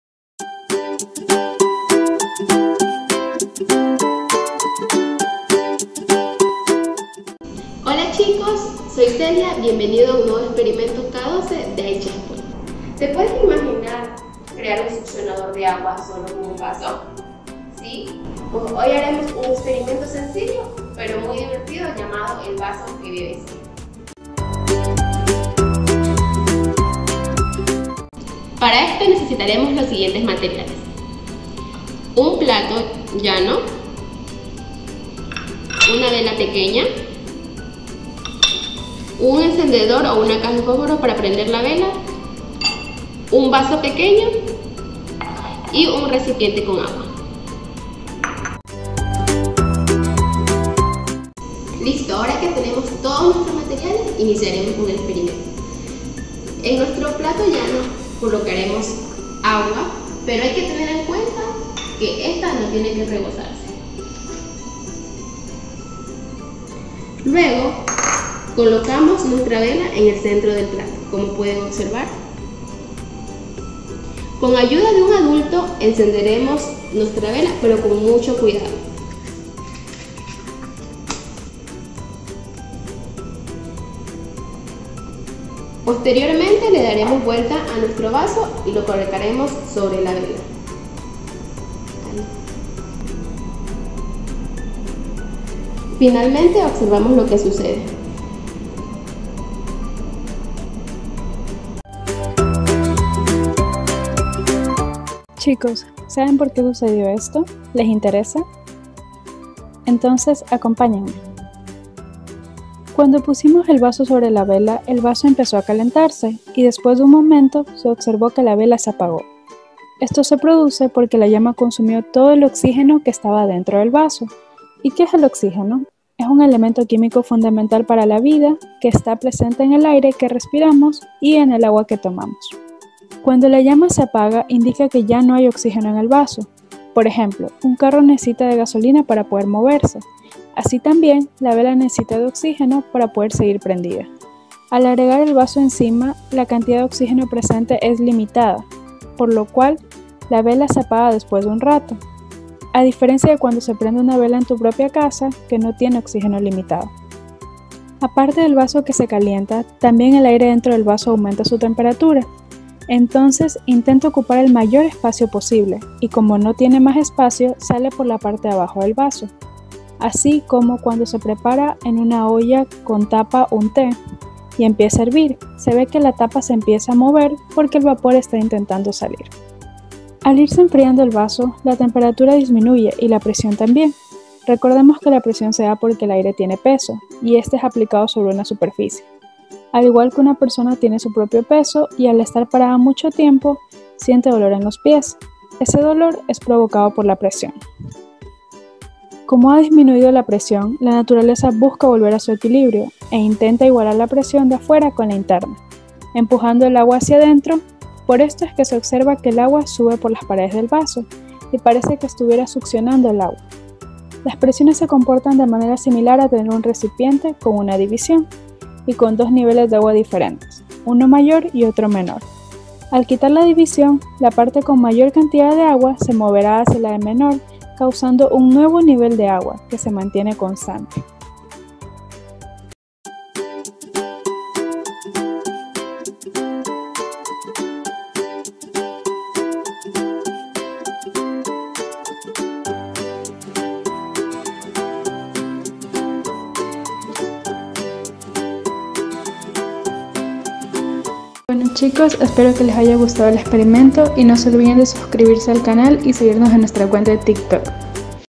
Crees que un vaso puede beber solo? 🥛Compruébalo con nosotros !!! 🌟
⏩ K-12 at AIChE ESPOL 🧪🎥

¿Creerías si te dicen que un vaso puede beber agua solo?🤔
Hoy te enseñaremos porque sucede esto mientras hacemos ciencia en el proceso 🧑🏻‍🔬

✅Cuéntanos tu experiencia y comenta con el vasobebedor